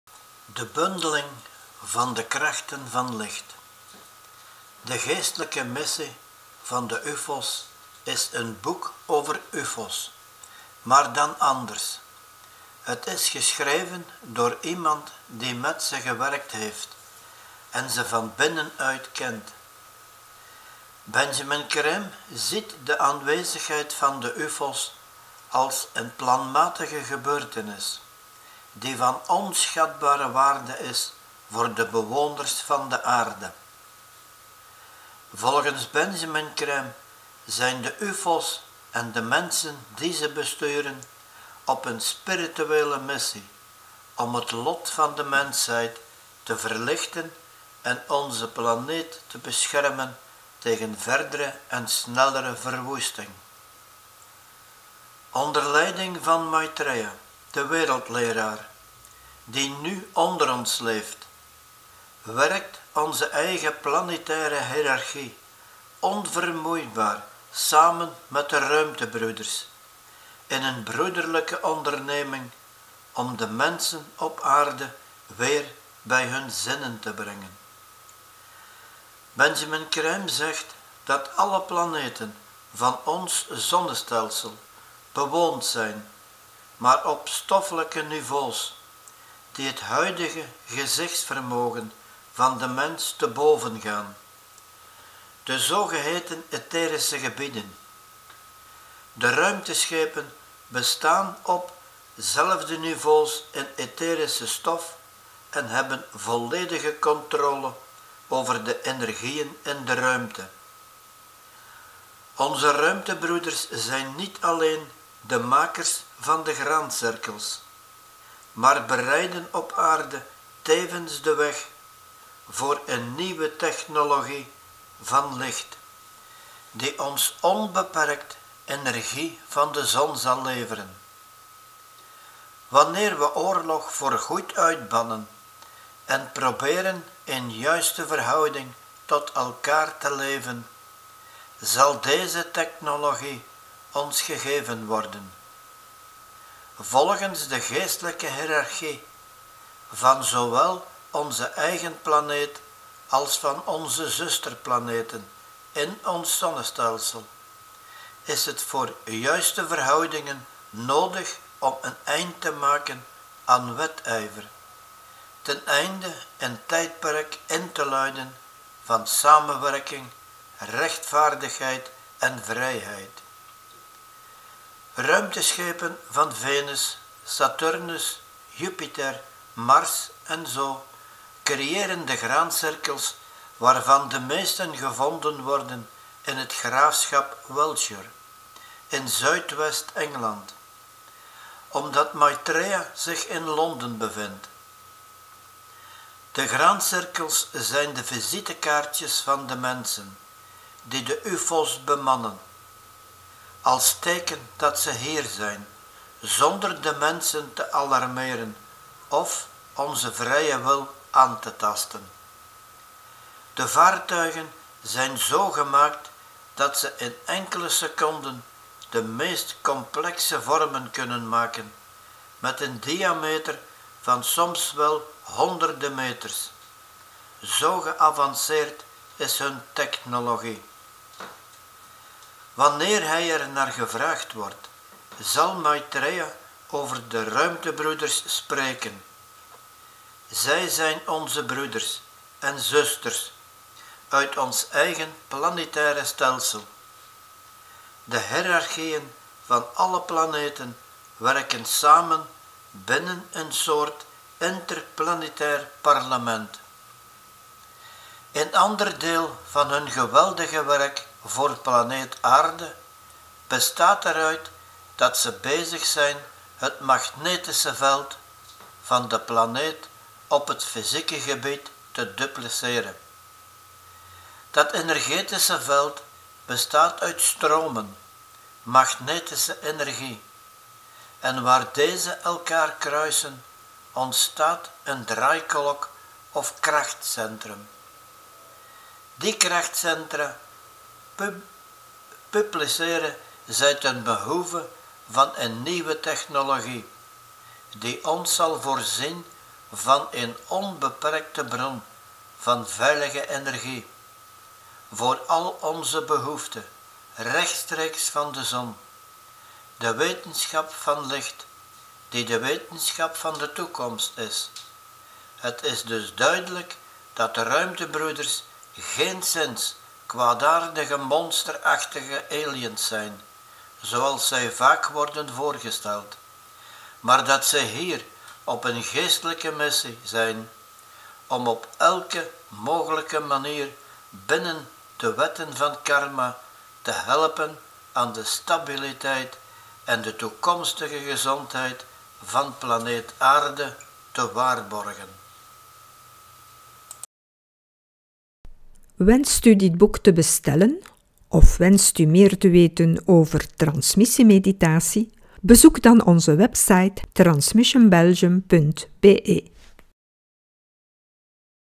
"De bundeling van de Krachten van Licht" - Benjamin Creme
Benjamin Creme (Glasgow, 5 december 1922 - Londen, 24 oktober 2016) was een Brits esotericus, kunstschilder, auteur, internationaal spreker en redacteur, die is uitgegroeid tot de voornaamste bron van informatie over de aanwezigheid van Maitreya, de Wereldleraar.
De Bundeling van de Krachten van Licht,  van Benjamin Creme,  is een boek over ufo’s, maar dan anders. Het is geschreven door iemand die zelf met de ufo’s heeft gewerkt en ze uit eigen ervaring kent. Benjamin Creme ziet de aanwezigheid van ufo’s als onderdeel van een plan en van onmetelijke waarde voor de mensen op Aarde.
Dit boek bestellen?   Meer weten over Transmission Belgium en Transmissiemeditatie?